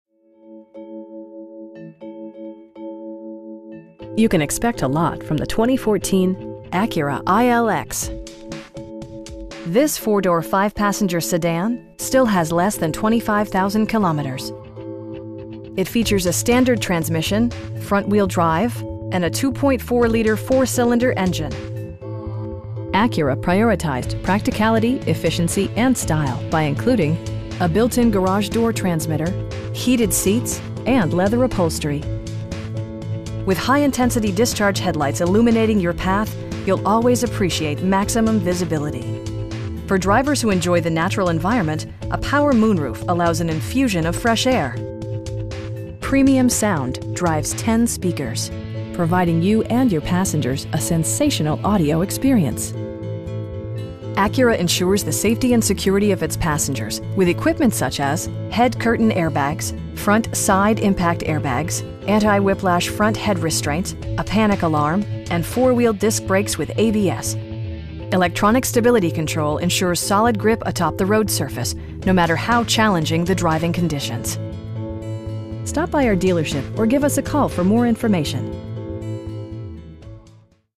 2014 Acura ILX in Dollard-Des Ormeaux, QC H9H 2A6
You can expect a lot from the 2014 Acura ILX. This 4 door, 5 passenger sedan still has less than 25,000 kilometers! It features a standard transmission, front-wheel drive, and a 2.4 liter 4 cylinder engine. Acura prioritized practicality, efficiency, and style by including: a built-in garage door transmitter, an outside temperature display, heated seats, tilt and telescoping steering wheel, power windows, cruise control, and leather upholstery. With high intensity discharge headlights illuminating your path, you'll always appreciate maximum visibility. For drivers who enjoy the natural environment, a power moon roof allows an infusion of fresh air. Premium sound drives 10 speakers, providing you and your passengers a sensational audio experience. Acura ensures the safety and security of its passengers with equipment such as: head curtain airbags, front side impact airbags, traction control, brake assist, anti-whiplash front head restraints, a panic alarm, and 4 wheel disc brakes with ABS. Electronic stability control ensures solid grip atop the road surface, no matter how challenging the driving conditions. We have the vehicle you've been searching for at a price you can afford. Stop by our dealership or give us a call for more information.